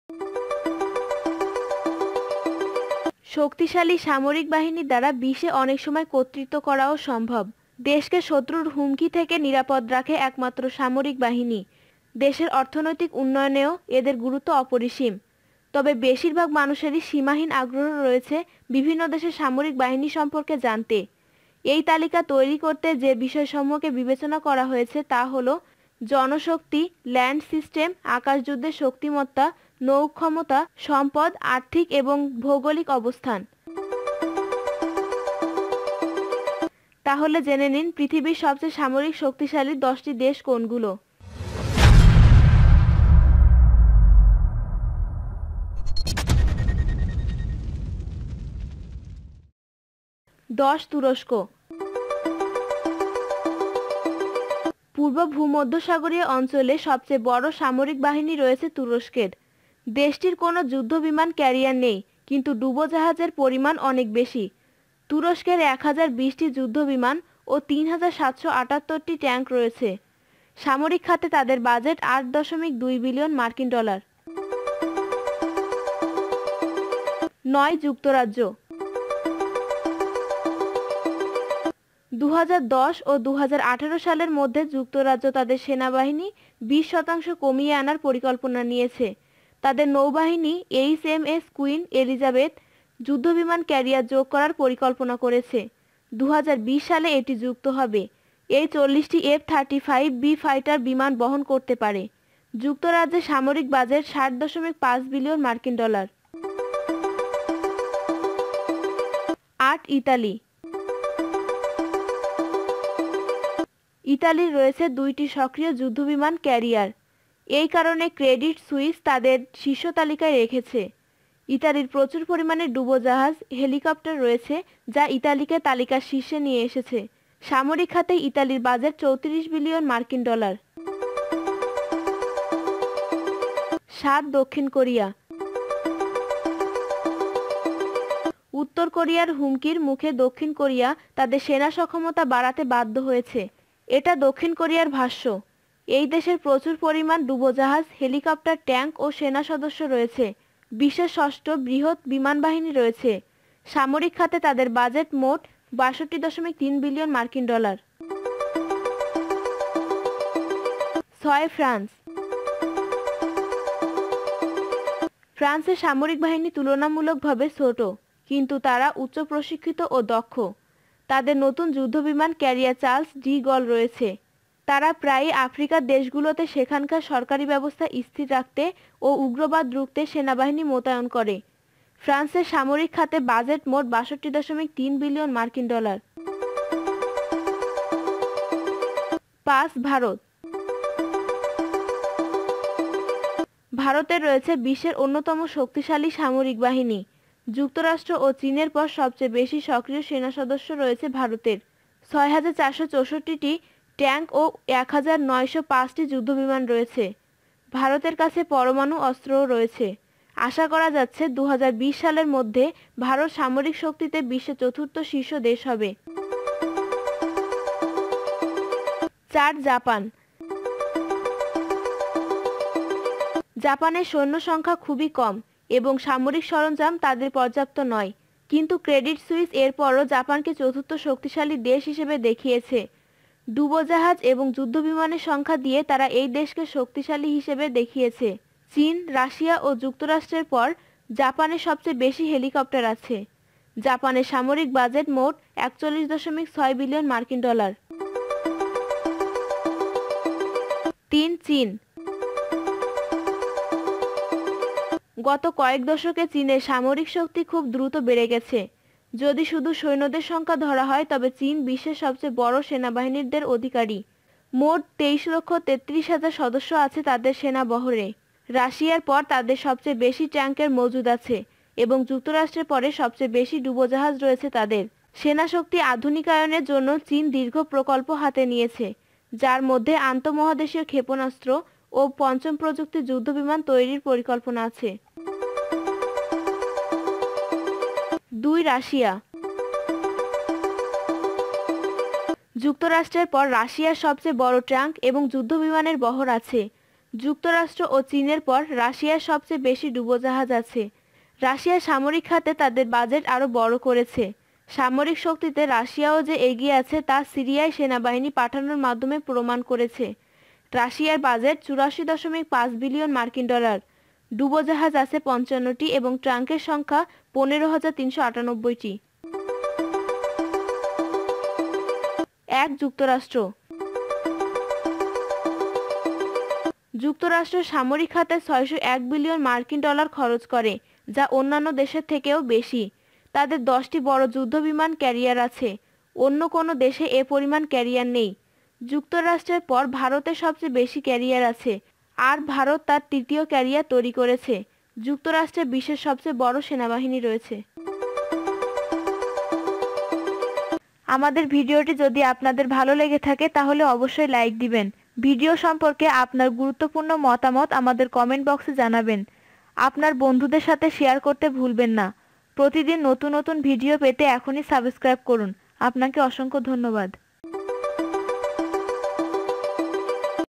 বিশ্বের সবচেয়ে সেরা শক্তিশালী ১০টি দেশ ।। Top 10 Most Powerful Countries in the World
শক্তিশালী সামরিক বাহিনীর দ্বারা বিশ্বে অনেক সময় কর্তৃত্ব করাও সম্ভব। দেশকে শত্রুর হুমকি থেকে নিরাপদ রাখে একমাত্র সামরিক বাহিনী, দেশের অর্থনৈতিক উন্নয়নেও এদের গুরুত্ব অপরিসীম। তবে বেশিরভাগ মানুষেরই সীমাহীন আগ্রহ রয়েছে বিভিন্ন দেশের সামরিক বাহিনী সম্পর্কে জানতে। এই তালিকা তৈরি করতে যে বিষয় সমূহকে বিবেচনা করা হয়েছে তা হলো, জন শক্তি, ল্যান্ড সিস্টেম, আকাশ যুদ্ধে শক্তিমত্তা, নৌ ক্ষমতা, সম্পদ, আর্থিক এবং ভৌগলিক অবস্তান। তাহলে জেনে নিন পৃথিবীর সবচেয়ে সামরিক শক্তিশালী ১০টি দেশ কোনগুলো

বিস্তারিত জানতে এখানে ক্লিক করুনঃ